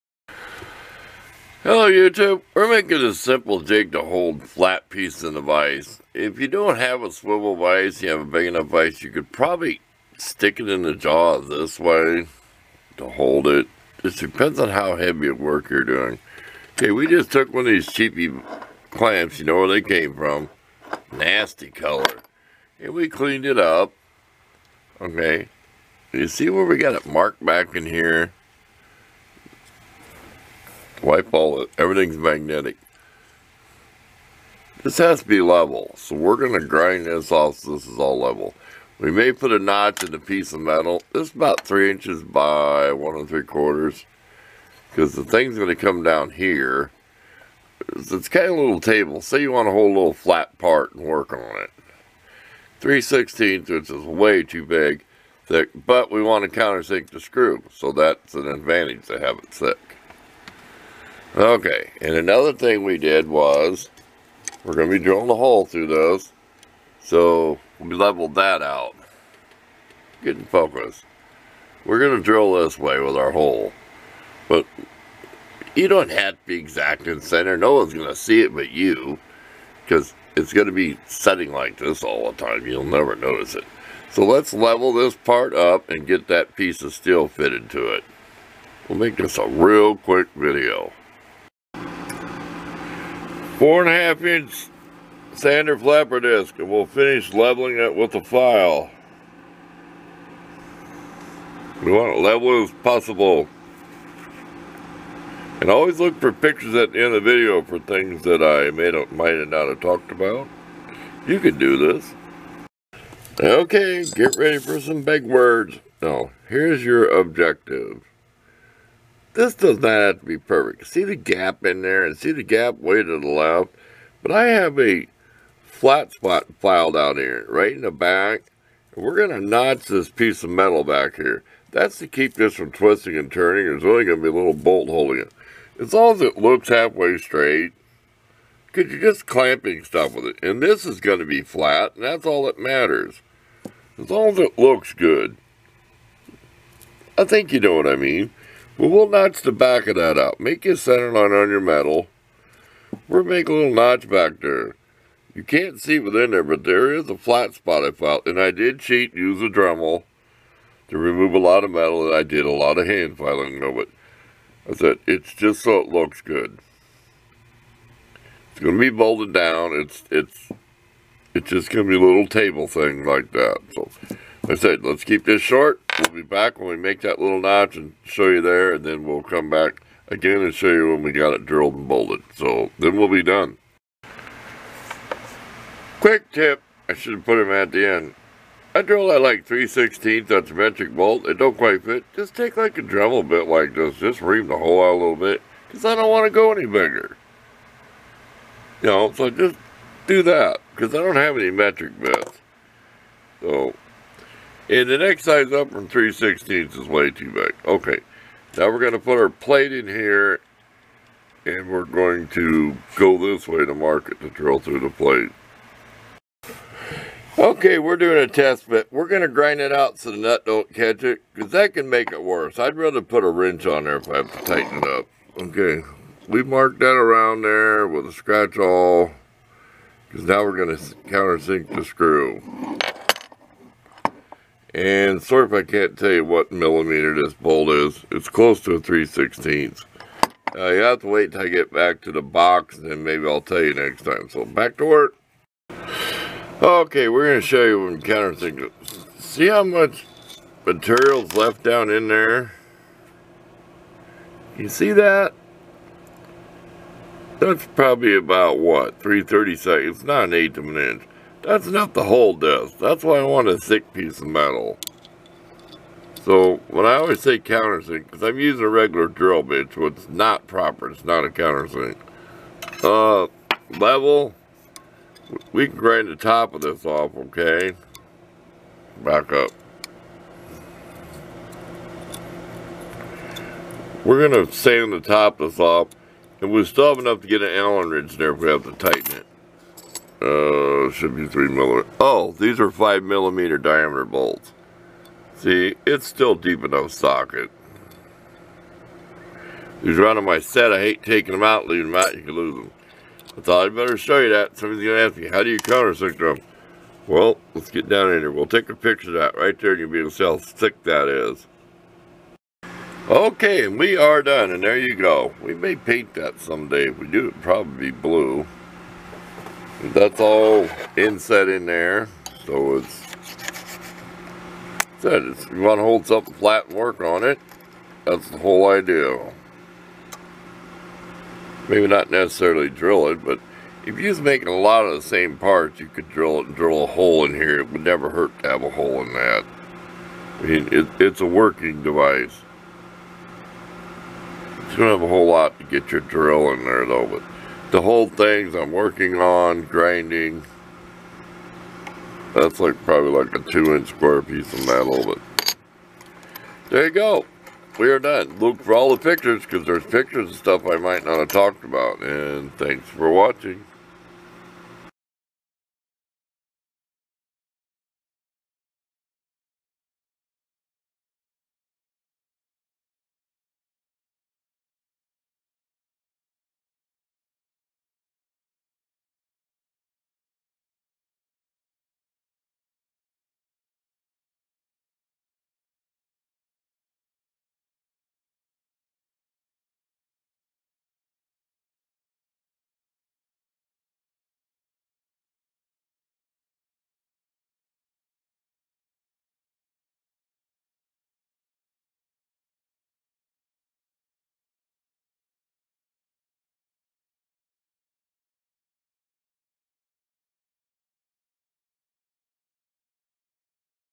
Homemade Clamping Jig made from 2 inch C clamp
Used for hold small pieces when drilling cutting etc.